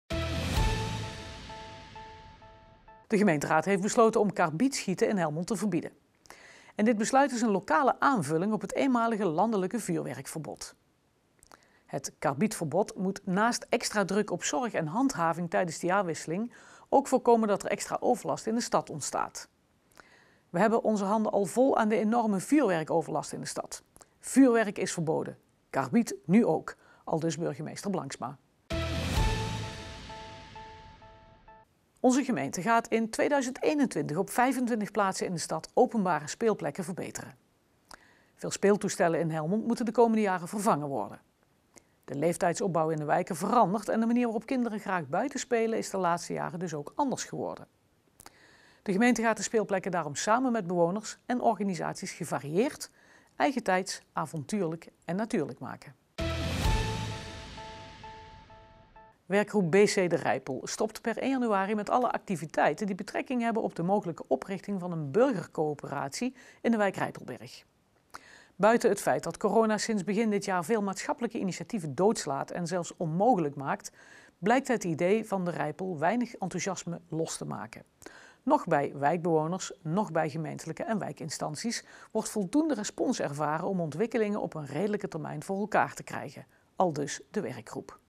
Het korte weekoverzicht van week 49 | Helmond
In het weekoverzicht van week 49: de gemeente heeft besloten om ook carbid schieten in Helmond te verbieden. De komende jaren gaan er in Helmond zo'n vijfentwintig speelplekken vernieuwd worden.

Verder nog: BC de Rijpel met de activiteiten per 1 januari 2021.